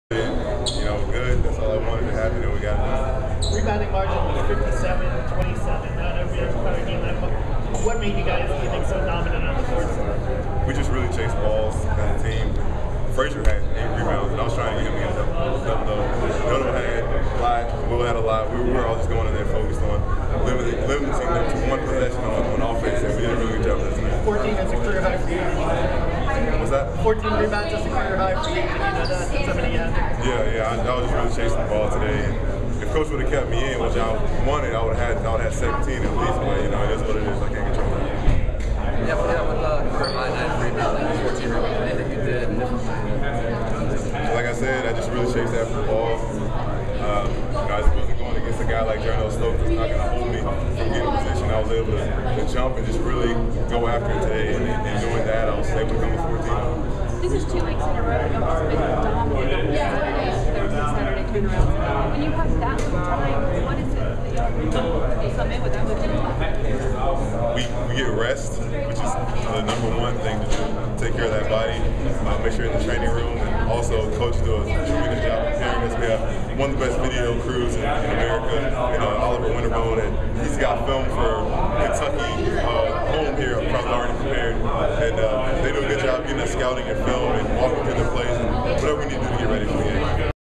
Patric Young on Texas A&M win
Florida center Patric Young talks about a 69-36 win over TAMU.